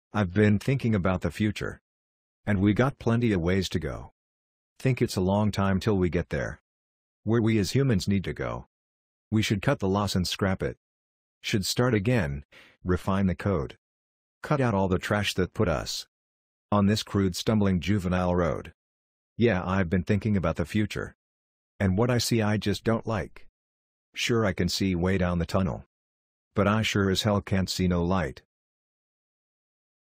Where is the Human Race headed
An iClone Animation discussing the bleak future we as Humans face, in the uncertain Political, and Economic Climate we now inhabit. Where is the Human Race headed is a personal observation, but one shared with many.